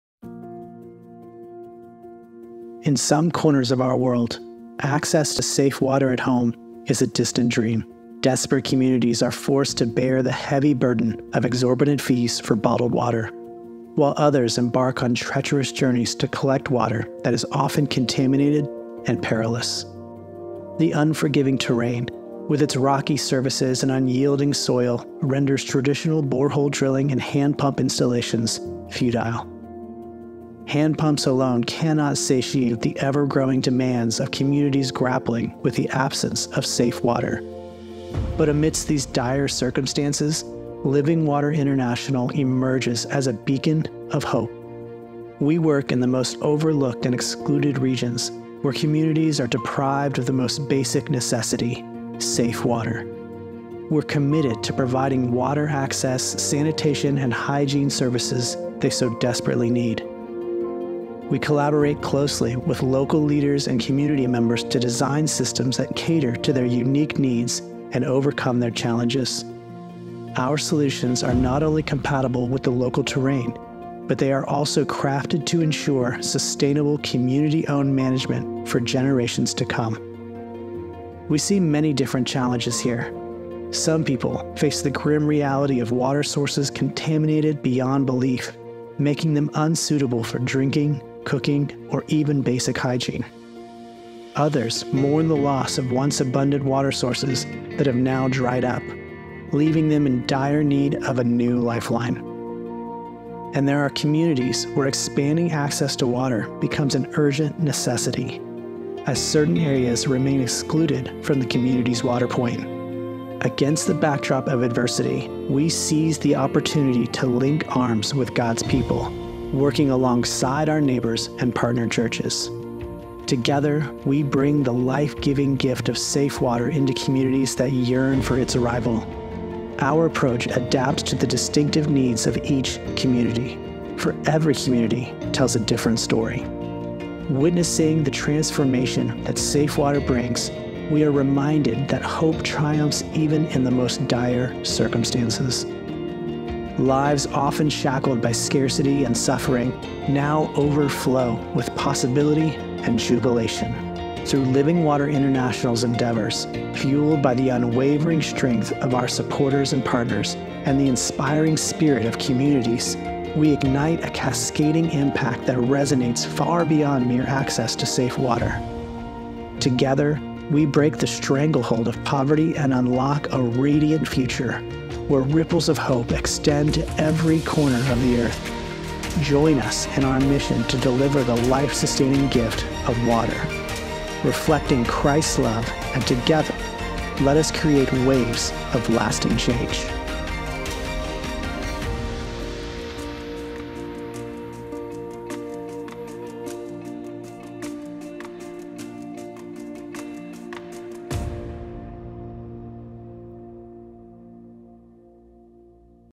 Piped Water Systems in Mexico
A look at some of the ways Living Water International goes beyond the well in developing unique solutions to fit each community in which we work.